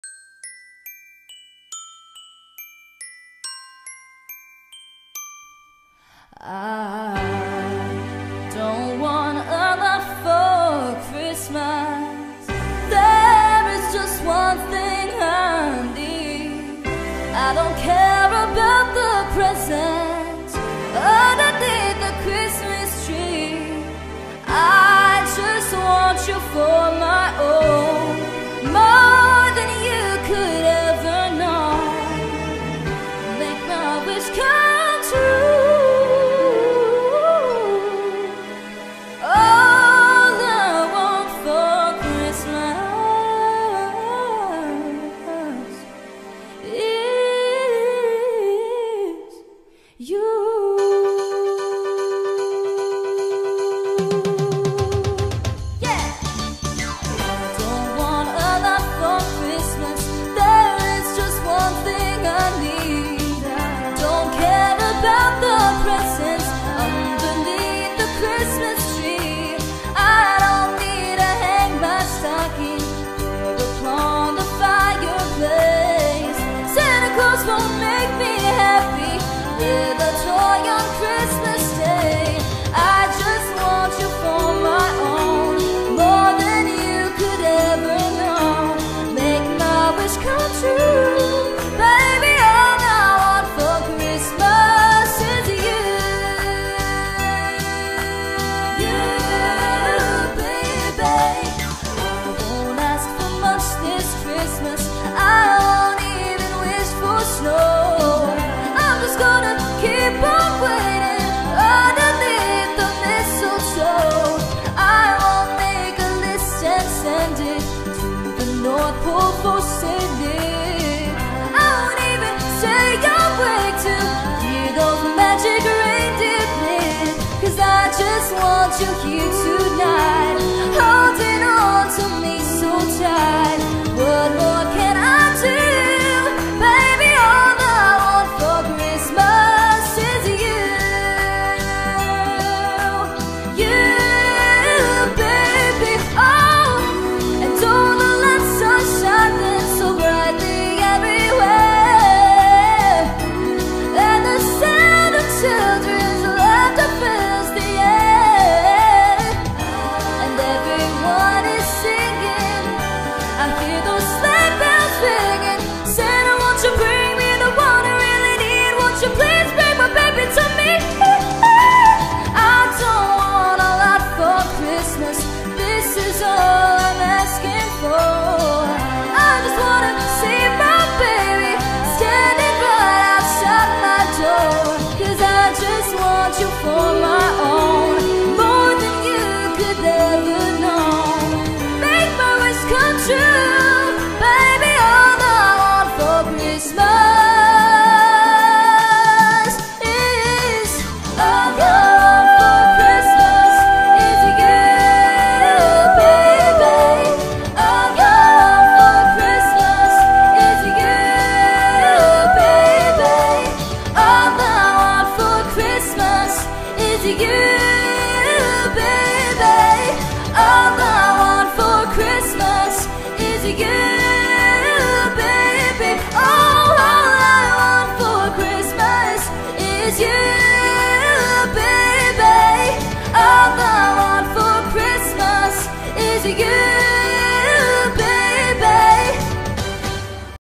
gieschristmasymep  CHRISTMAS MEP  🎄
thank you for everyone’s hard work to make this MEP sorry for being unorganized with this MEP, it’s my first time
I had to mix some of you guys parts for some reasons, and don’t get mad that I posted this in January, it’s still holiday season 😞

SPECIAL THANKS TO THESE PEOPLE FOR THEIR HARD WORK
@nyowisxandermatthewskisser @Zenith @CryptidHunter_ @aIexbrightman @JstBlake @Marzie @[•taeluvs•] @soggylivers @annieyki138  @L1ME